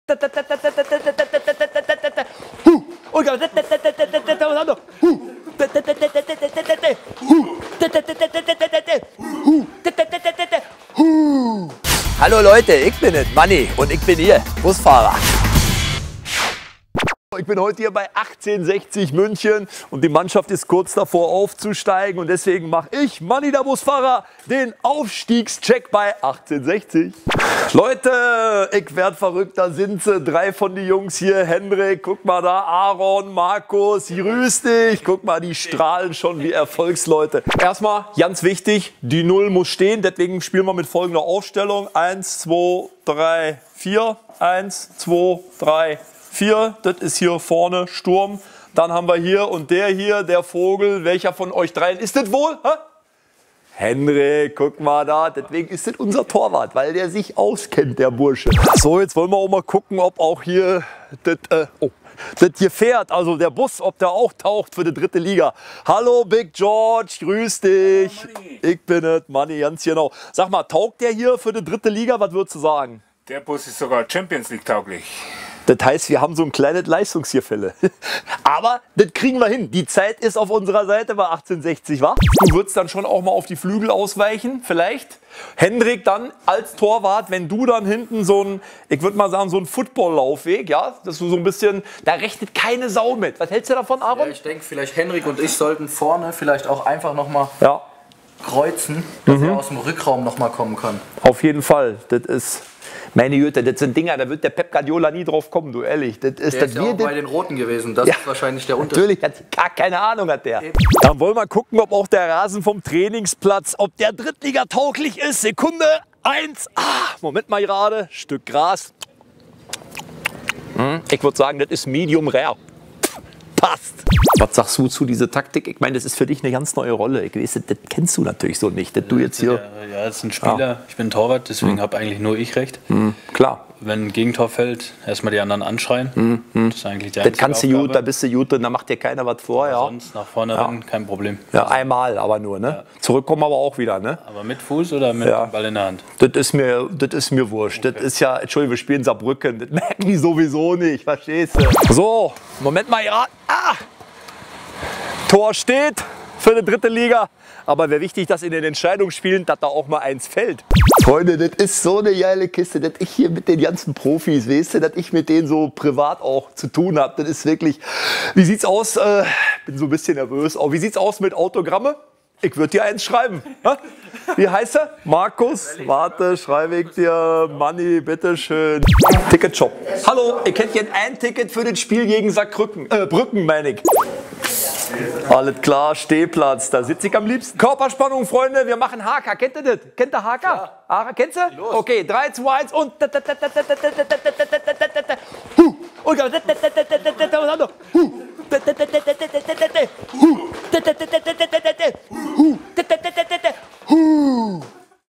Aufstiegs-Check: Matze Knop nimmt 1860 München unter die Lupe
Der TSV 1860 München trifft in der Relegation um den Aufstieg in die 3. Liga auf den 1. FC Saarbrücken. Vor den beiden Entscheidungsspielen hat sich „MANni der Busfahrer“ bei den Löwen umgeguckt. Sein Fazit: Der Verein ist definitiv reif für höhere Aufgaben. Aber überzeugt Euch einfach selbst.